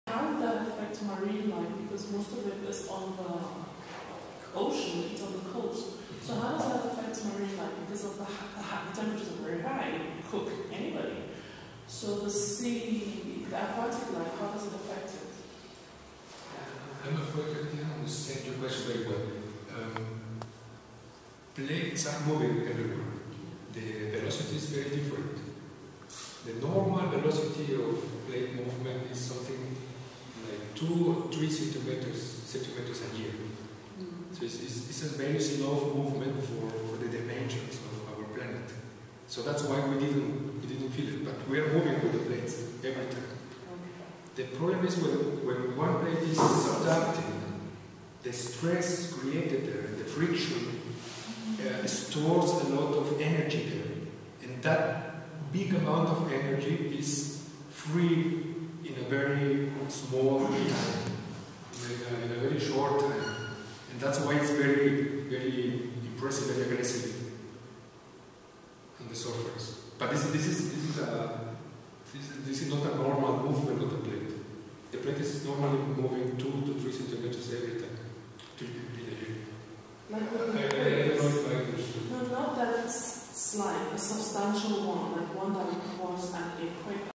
Another excerpt from Ignacio Torres' overview of Geothermal Energy (12th April 2011)